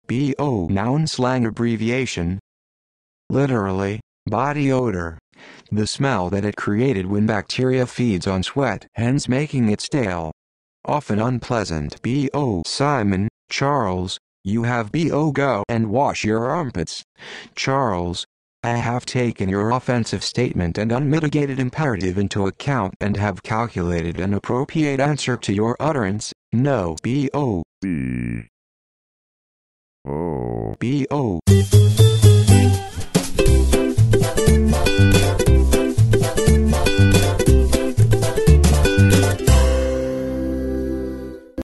B.O. meaning and pronunciation
≺noun≻ Slang abbreviation;

Literally, Body Odour. The smell that it created when bacteria feeds on sweat hence making it stale. Often unpleasant.

B.O. definition by Urban Dictionary